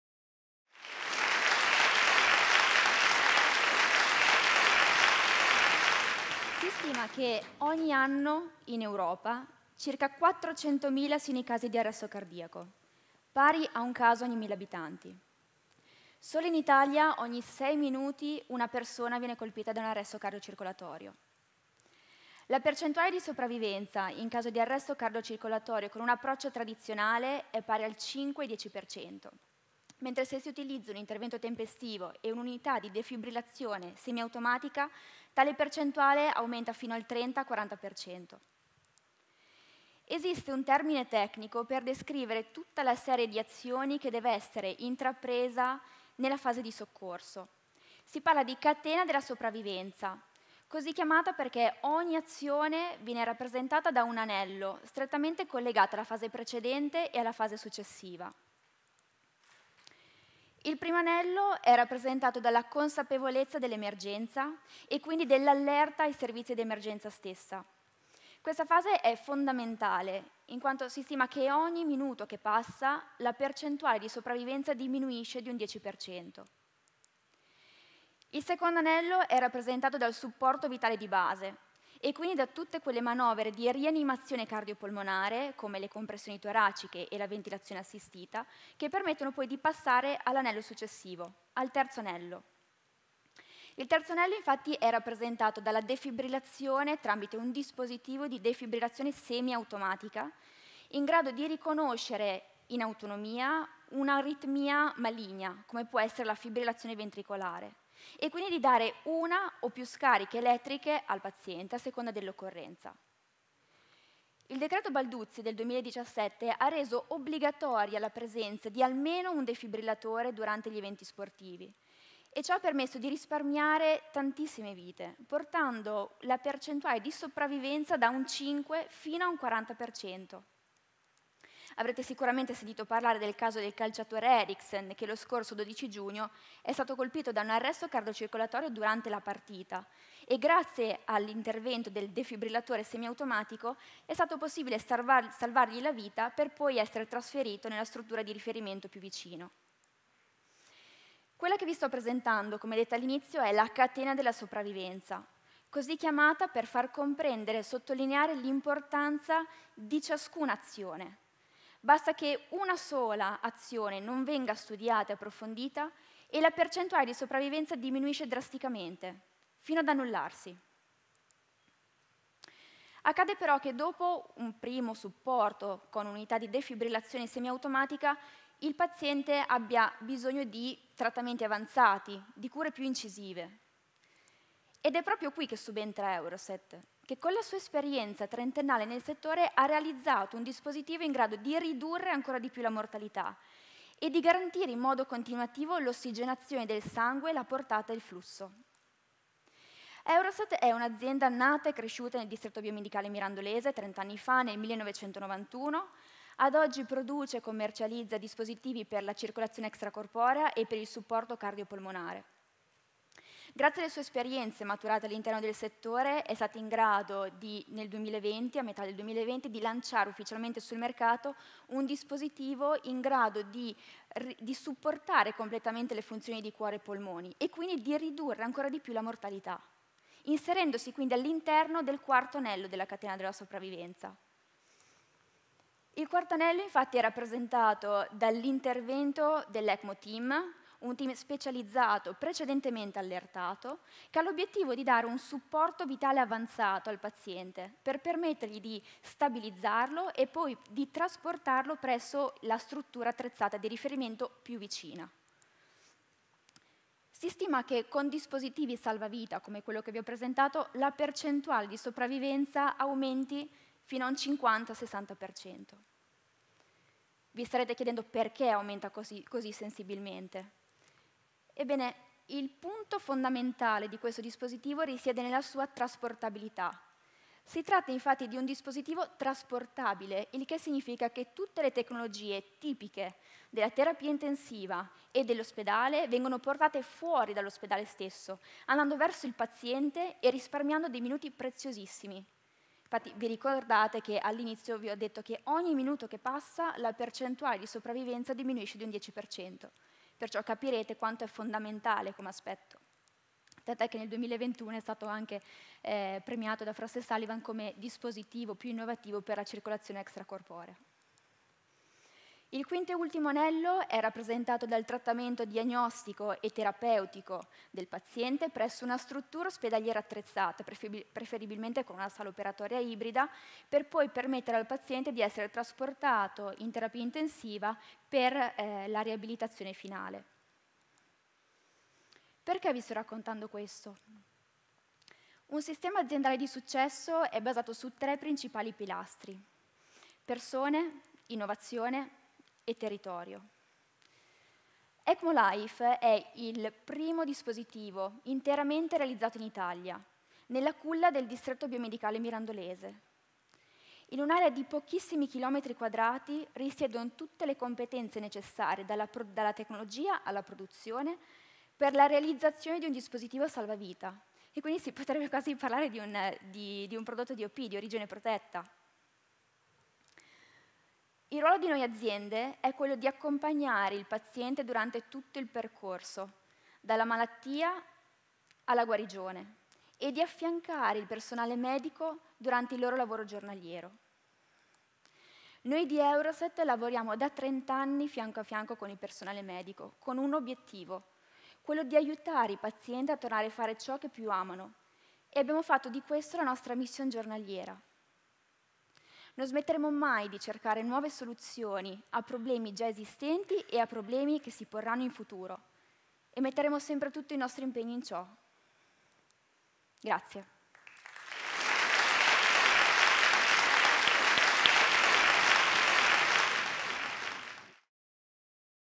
Eurosets | Sara Menghini | Biomedical valley
This talk was given at a Biomedical valley workshop event held in Mirandola (MO) Italy on July 2021.